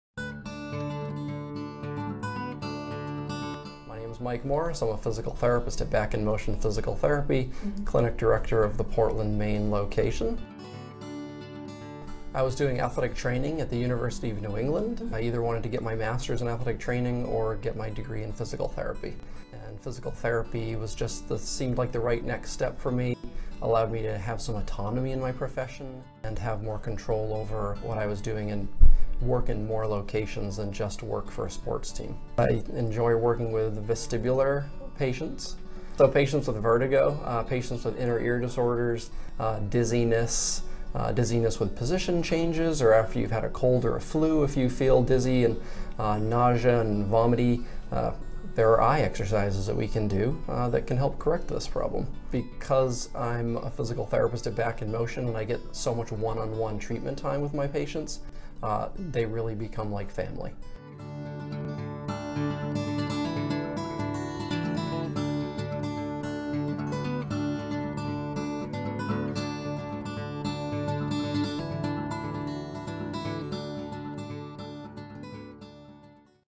Physical Therapist Michael Moras
Meet Back in Motion Physical Therapist Michael Moras of our Portland, Maine clinic. Mike specializes in dry needling, treating vertigo, and helping patients with prosthesis.